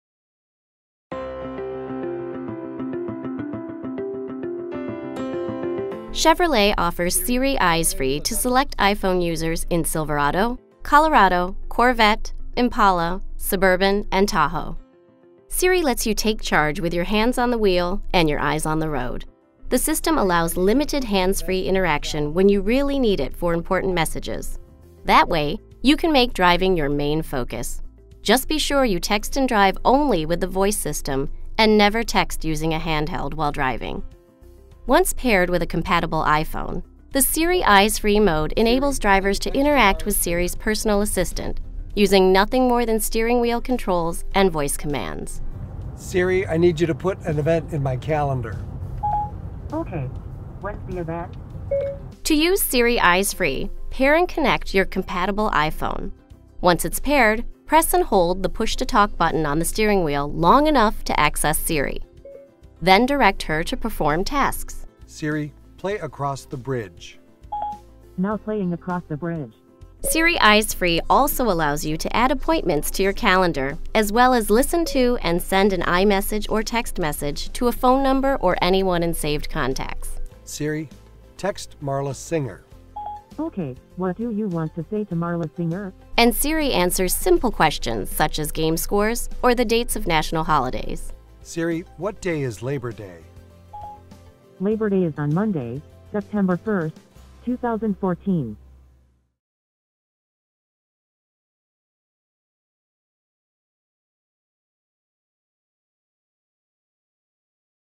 Chevy MyLink 8" with Siri Eyes Free
See how the Chevrolet MyLink system with SMS Texting and Siri Eyes Free, help keep you safe and focused on the road.